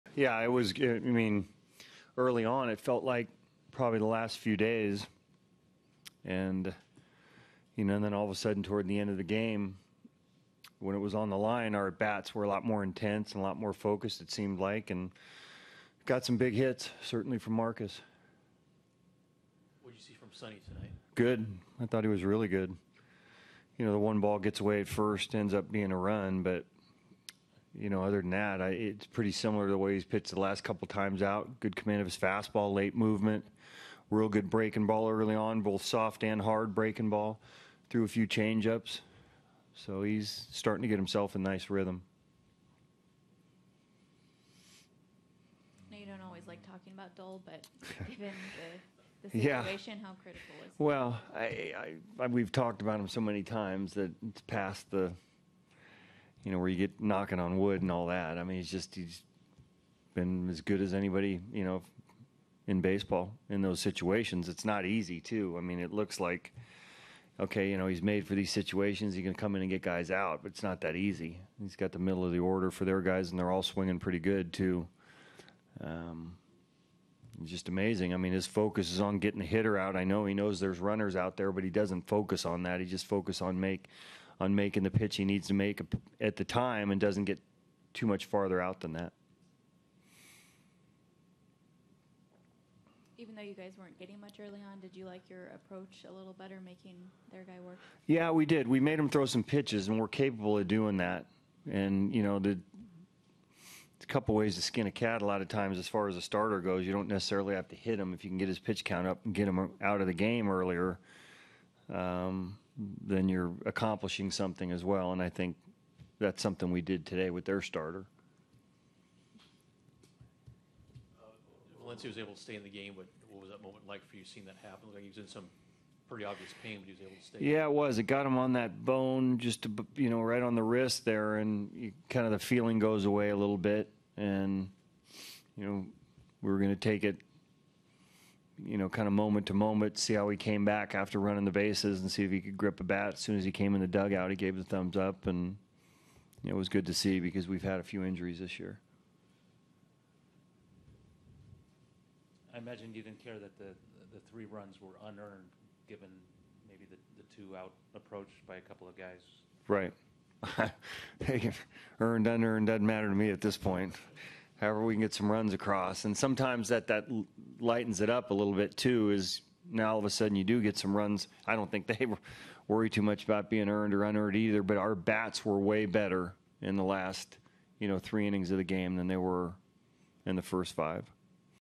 OAK@MIL: Melvin discusses plate discipline in win
Athletics manager Bob Melvin discusses how his club made the opposing pitchers work in a win over the Brewers